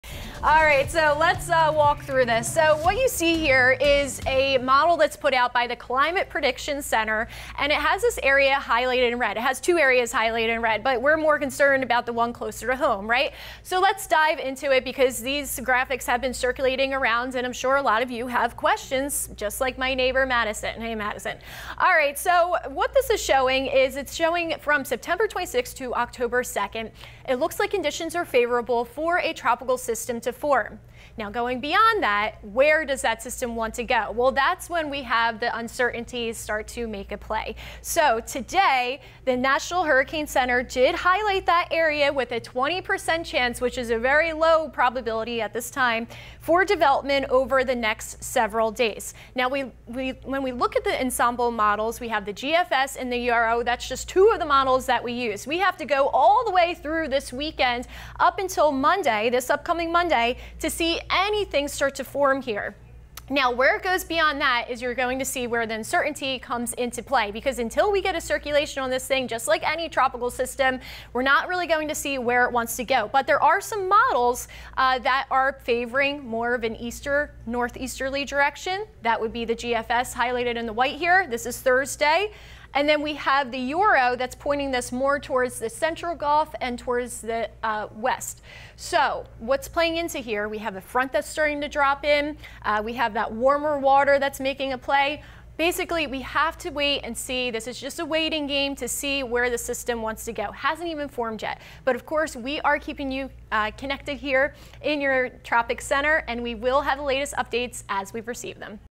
Tracking the Tropics: Potential tropical system likely to form in Gulf next week, while Gordon could
There are two different areas we are monitoring for the possibility of tropical development.

The first disturbance is in the central Atlantic and will likely become a fish storm, which means it will not impact any landmass. It includes remnants of Gordon, which was the latest tropical storm to form. A tropical depression or storm could re-form in a few days. There's a 50 percent chance for development over the next seven days, with a 20 percent over the next two days.

The second area of development possible is in the Caribbean Sea.

Right now, a slow-moving disturbance has a low chance over the next week of forming a low-pressure system over the northwestern Caribbean. There's no immediate development chance and it remains at 20 percent over the next seven days.

However, we need to watch this portion of the Caribbean closely because of something called the "Central American Gyre," which starts to become active in the late spring into the early fall.

The Central American Gyre, a large area of weak, low pressure, is active over Central America. This system can occasionally spin off more areas of compact, low pressure that can eventually become tropical storms or even hurricanes.